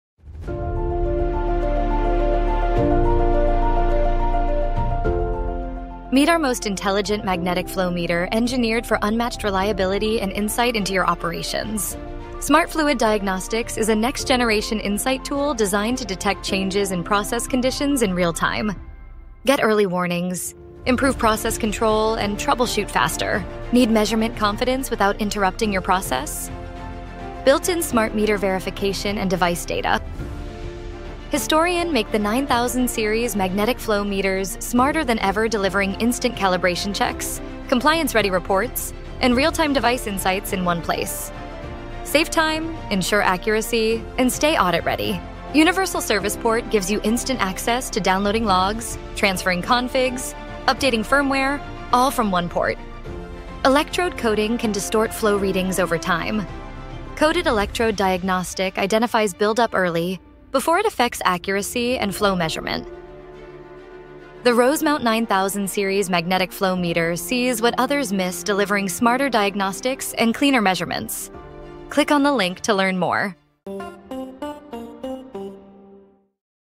Rosemount™ 9000 Series Magnetic Flow Meters | Most Intelligent
Explore why the Rosemount™ 9000 Series™ Magnetic Flow Meter is our most intelligent device yet. From Smart Fluid Diagnostics and Smart Meter Verification to Coated Electrode Detection and continuous Device Data Logging, you'll see how it delivers accuracy, reliability, and insight that help you stay ahead every day.